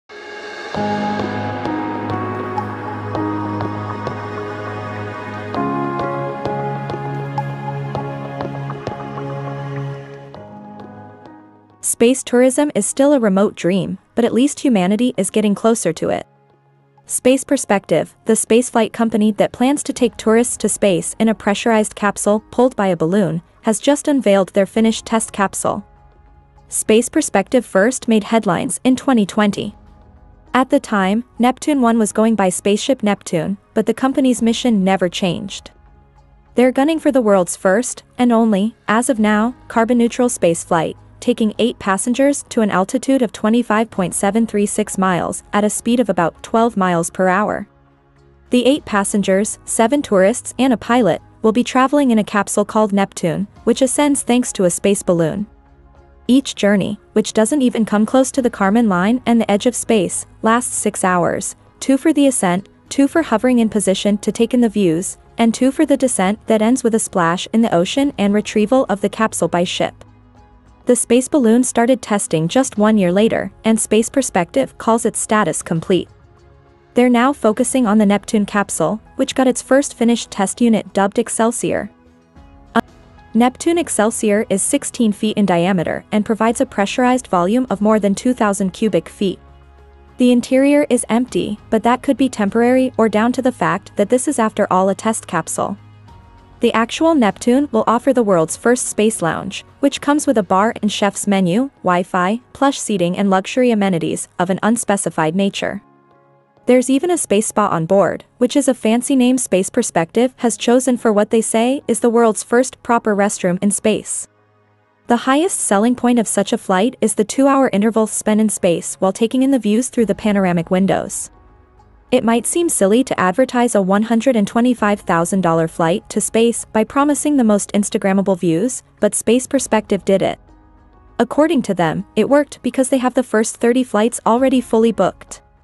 This Luxury Capsule Flies People to Space by Balloon
Space Perspective revealed its first completed test capsule called Excelsior with its 360 degree panoramic windows to look down on Earth and up into the night sky. the capsule provides a pressurized volume of 2,000 cubic feet and will allow up to eight passengers and a pilot to enjoy these views while comfortably sipping a beverage and it has Wi-Fi.